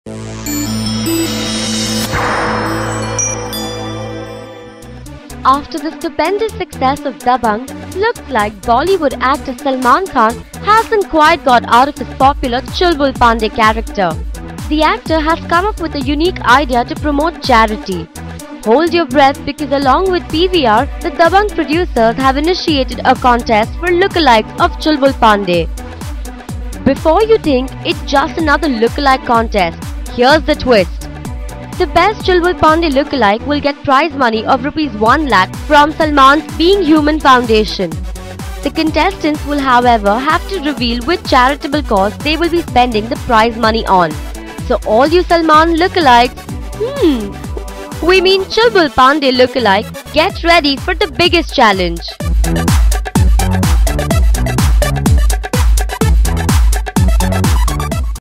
Salman wants 'Chulbul Pandey' look-alike!!
After the stupendous success of 'Dabangg', looks like Bollywood actor Salman Khan hasn't quite got out of his popular Chulbul Pandey character!!